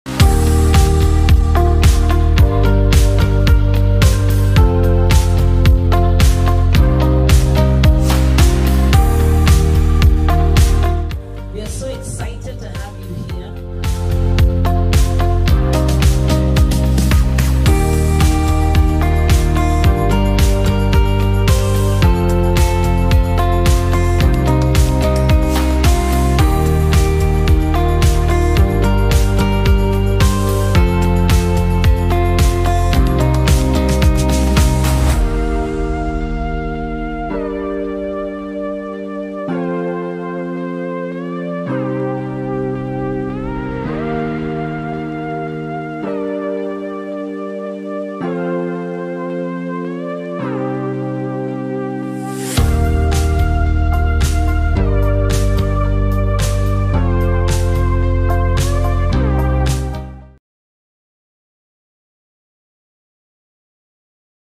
Evening Meet & Great with Fun Ice Breakers
A great member and greet facilitated by Adept Advisory with a blend of fun activities and energizers.